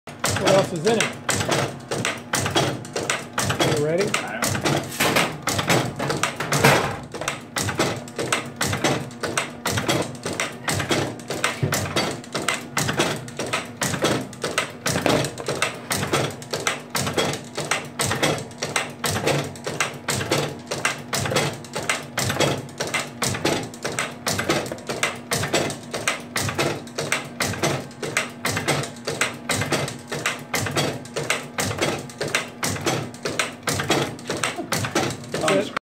Super Kookeking making granola
1" granola pieces with filler block die needed a little pressure on the dough